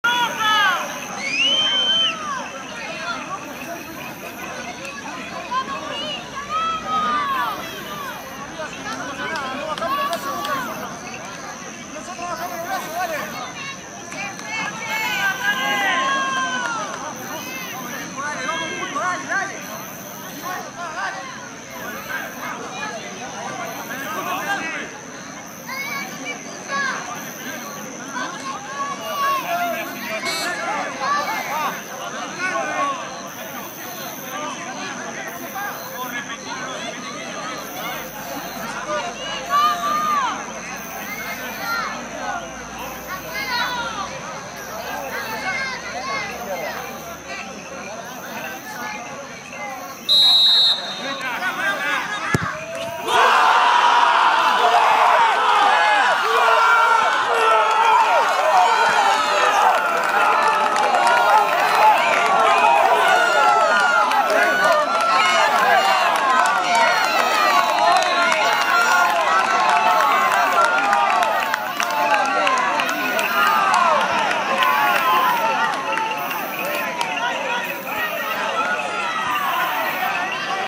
Indirecto.com.uy - Gol de Liga Mayor de Maldonado en Tala. Martín Nerón de penal
Maldonado Capital enfrentaba a Canelones del Este en la cancha del Club Nacional de Tala. La selección que dirige Eduardo Pelúa tenía que ganar si o si para poder seguir en carrera en el Campeonato del Este 2014. Iban 75' de un partido complicado. Derriban a Matías "Cocona" Pereira dentro del área y el árbitro sanciona penal. Lo ejecuta notablemente Martín Nerón y es goooollll!! Fue el único tanto del partido y le sirvió a Maldonado para avanzar a la siguiente fase donde enfrentará a Lascano, que representa a Rocha Interior.